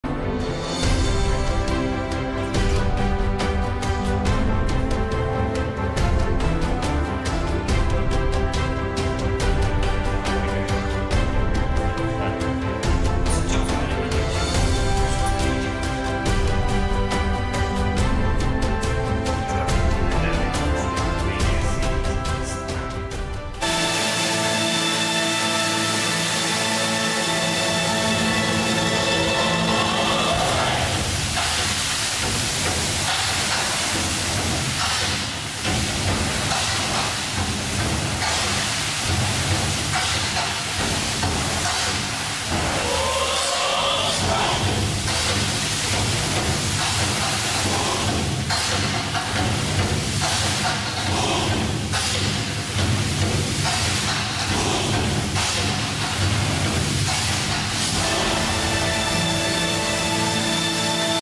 Syncronorm @ 2023 China Fountain & Waterscape Summit Forum
Syncronorm presents Depence R3 at the 2023 China Fountain & Waterscape Summit Forum

Under the slogan "Spring Moving China", this year's China Fountain & Waterscape Summit Forum was held in Yangzhou from March 23rd to 26th. Syncronorm is enormously proud to have been part of this unique conference, where leading companies of the Chinese fountain industry highlighted the future of the industry in exciting discussion panels and presentations.